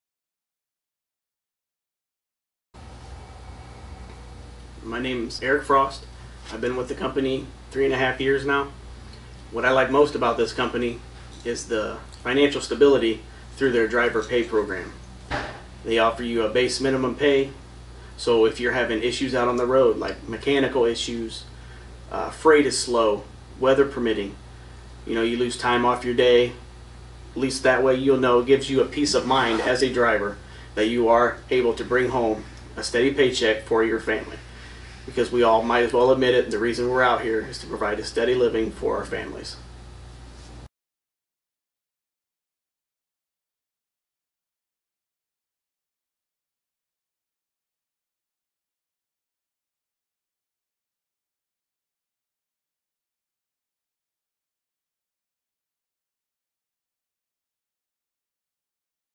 Keller Trucking | Financial Stability
Keller Driver, Eric Frost, gives his reasons for driving for Keller - Financial Stability